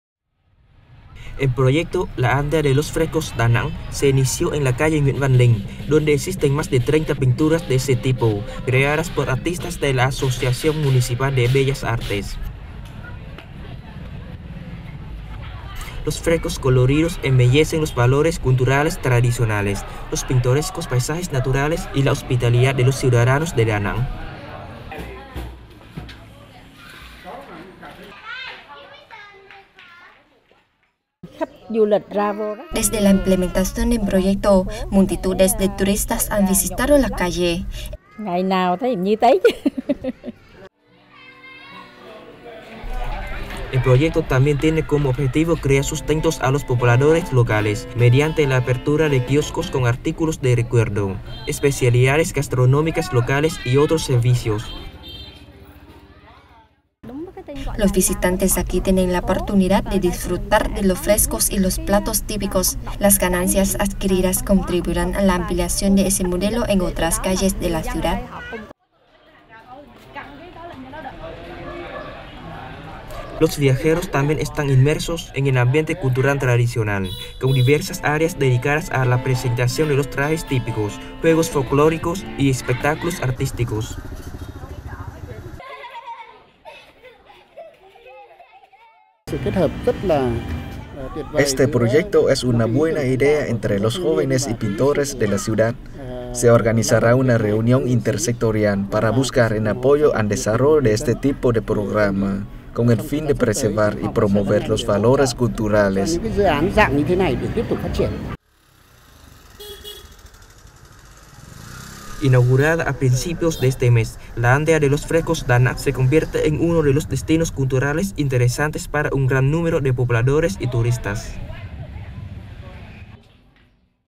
Ciudad de Da Nang desarrolla aldea de los frescos
Autoridades de la ciudad central de Da Nang despliegan un plan de desarrollo del modelo de “la aldea de los frescos” para convertirla en un sitio turístico interesante.